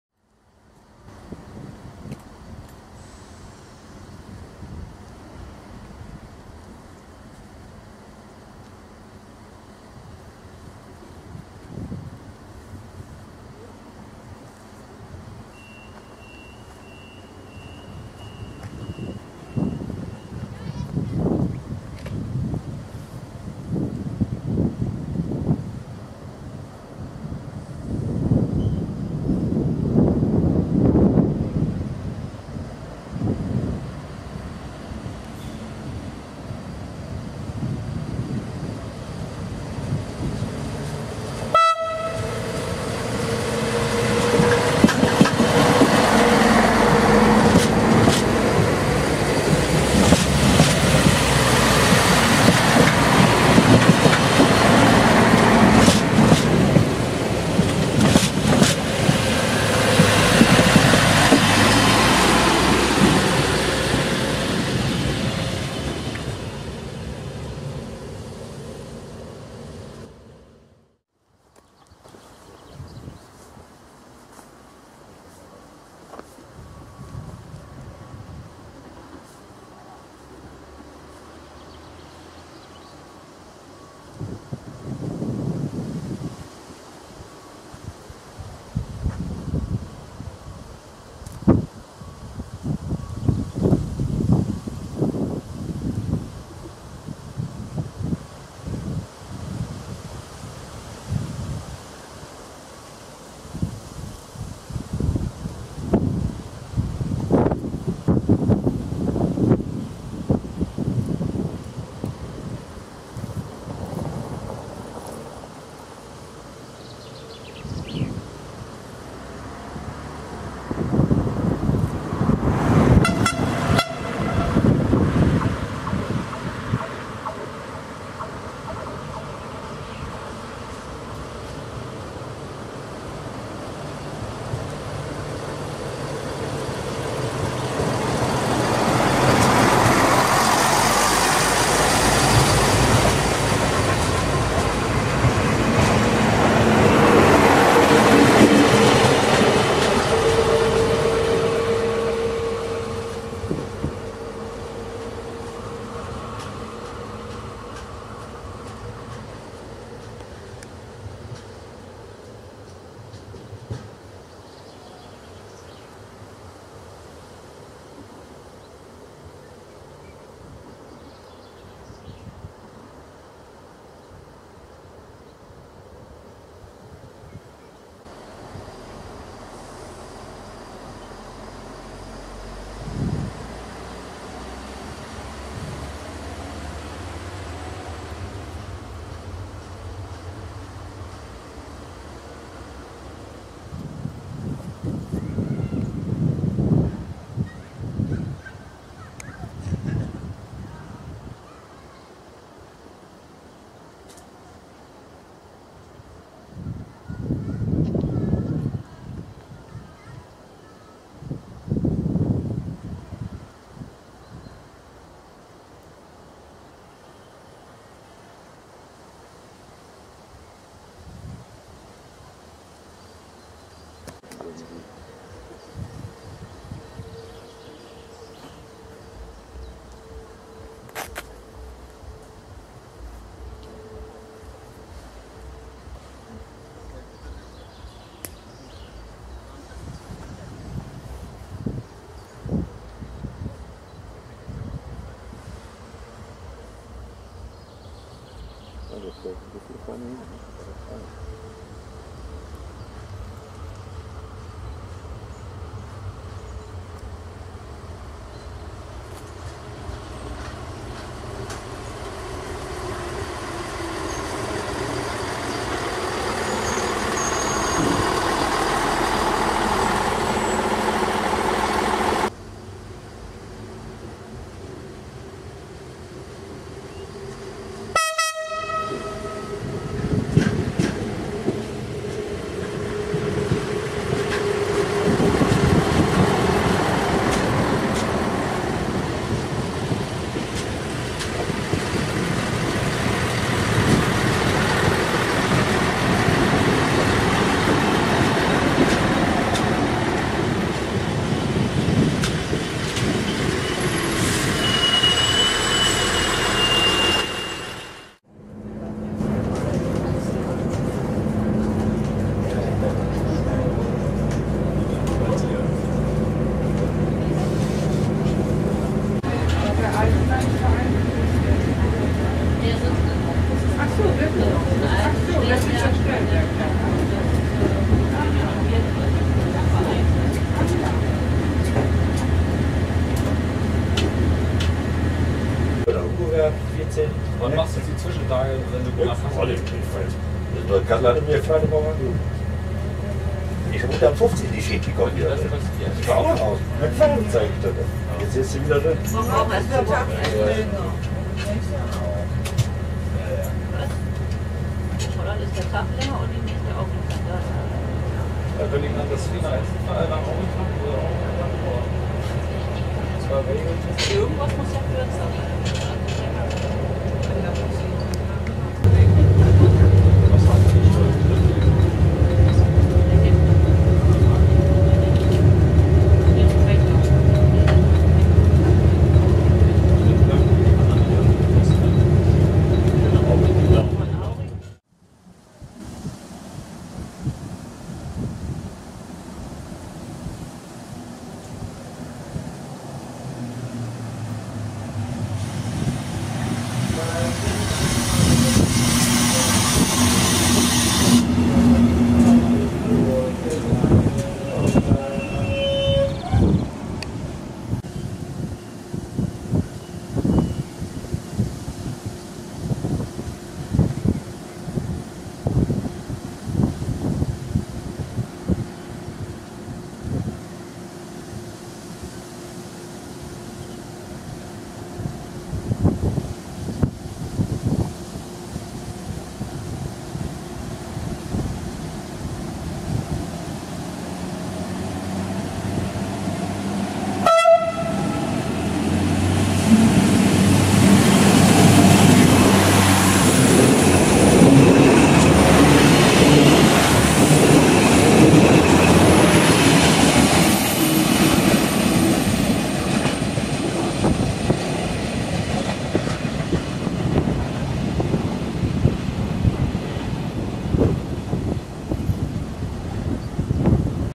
Personenverkehr zwischen Emden und Aurich zum Tag der Niedersachsen 2011
Zum 31. Tag der Niedersachsen, der vom 1. bis zum 3. Juli 2011 in Aurich stattfand, fuhren Sonderzügeder Arbeitsgemeinschaft Schienenverkehr Münsterland mit dem Namen "Ostfriesland-Express"  mit 643 072 und 073 der DB  zwischen Emden Hbf und Aurich.
Viele nutzten das Angebot, von oder nach Aurich mit dem Zug fahren zu können.
Vielen Dank an das sehr nette Team der ASM und der DB!